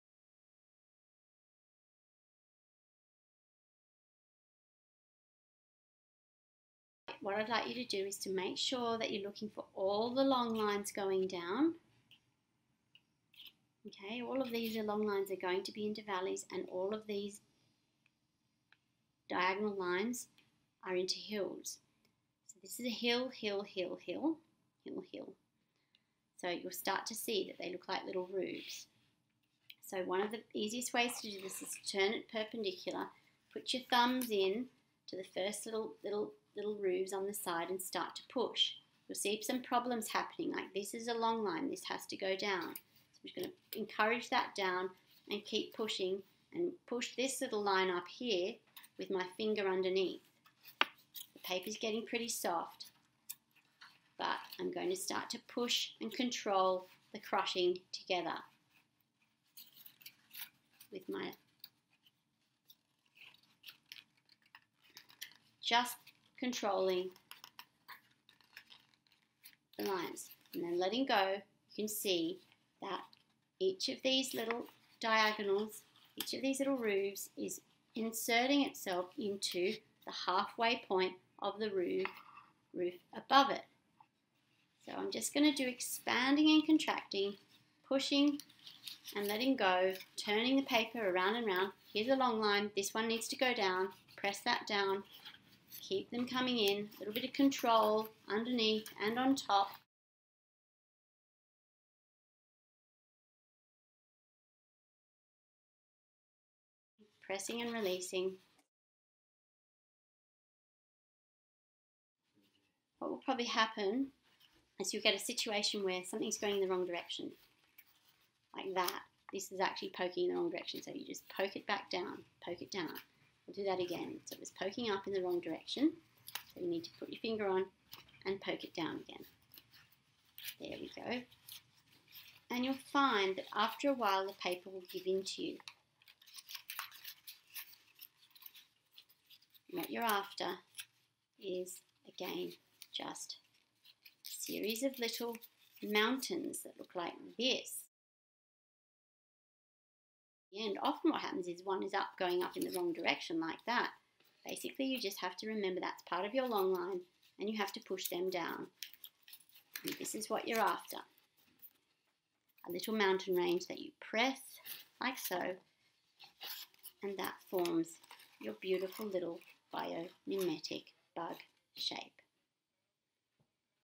STEAMPOP 3 folding the glide reflection bio bug
Constructing the paper into the biomimicry bug shape.